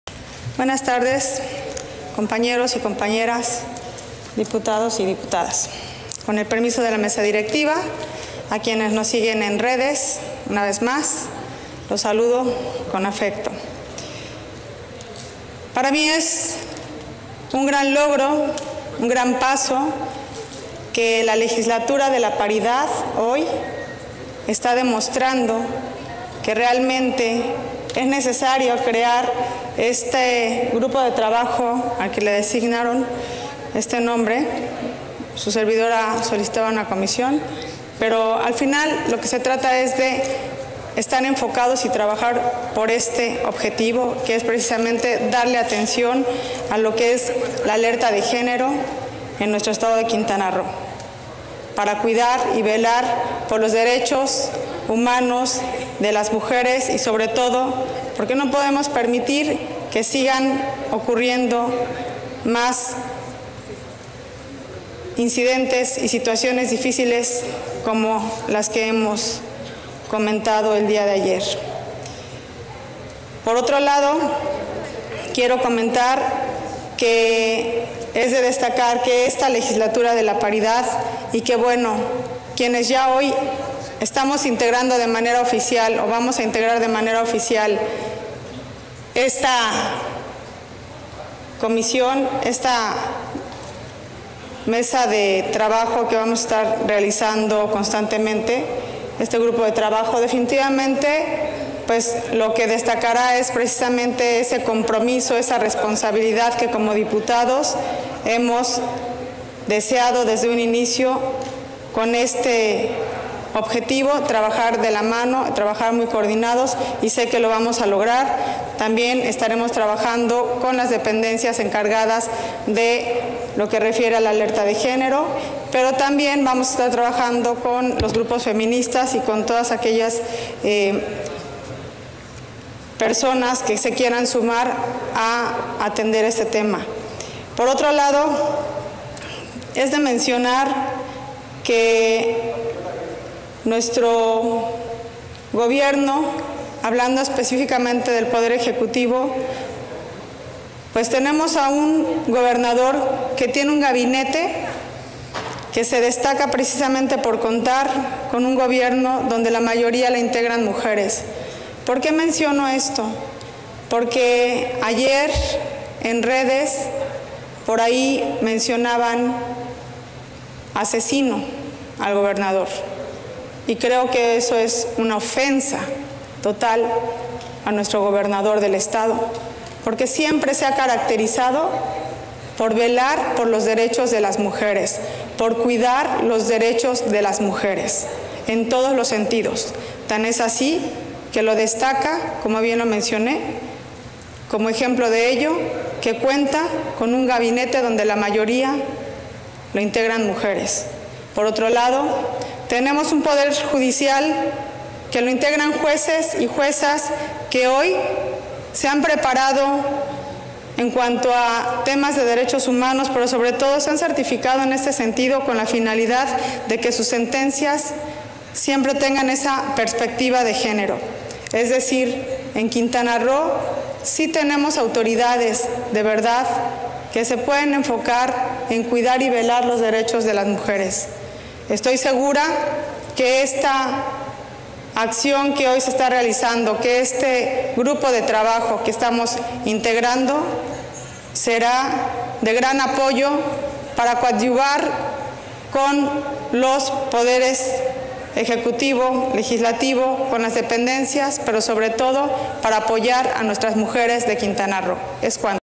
Intervención de la Dip. Roxana Lili Campos Miranda. PAN (11/Nov/2020)
Intervención de la Diputada Roxana Lili Campos Miranda, del PAN, en relación al Acuerdo de la Junta de Gobierno y Coordinación Política de la H. XVI Legislatura del Estado Libre y Soberano de Quintana Roo, por el que se aprueba la creación del Grupo de Trabajo para la atención de la Alerta de Violencia de Género contra las mujeres en Quintana Roo, del Poder Legislativo del Estado de Quintana Roo.